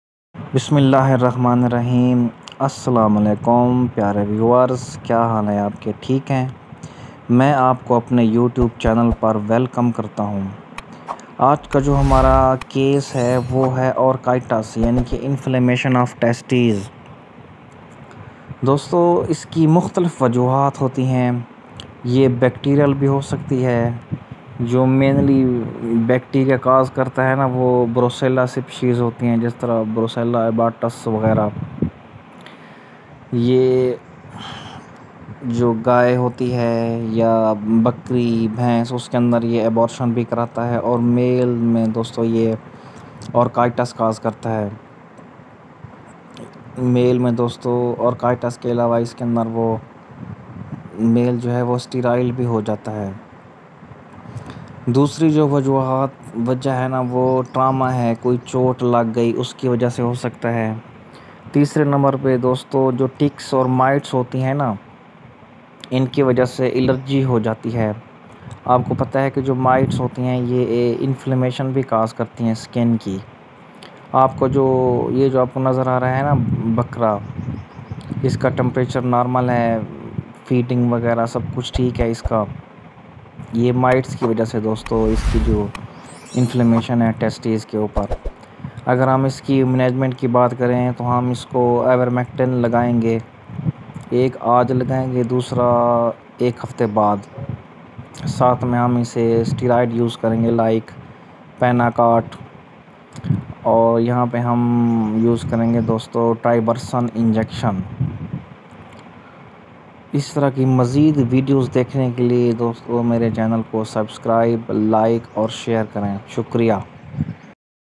Orchitis l Inflammation of testes in goat l Shahzad Drishak
Orchitis l Inflammation of testicles in goat l testes problem l testes wound Dr Shahzad Drishak